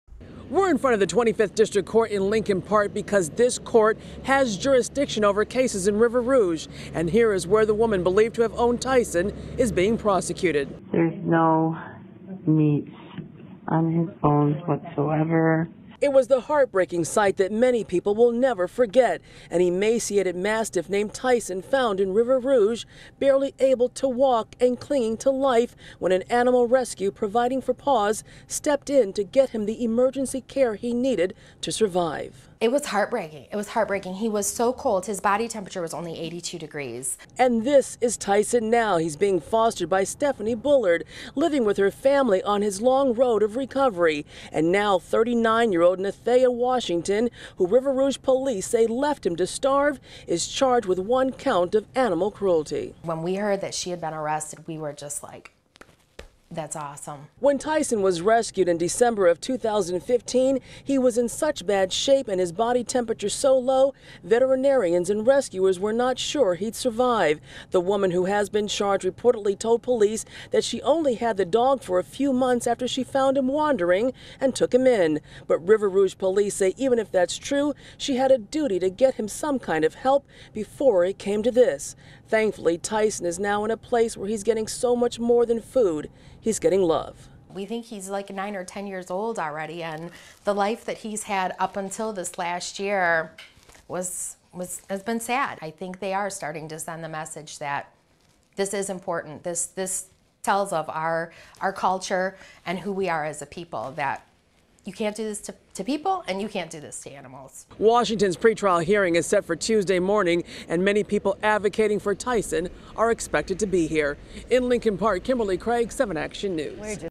Rescued dog gets a new leash on life
7 Action News Reporter Kimberly Craig gives us a update on a rescued dog, originally found starved and near death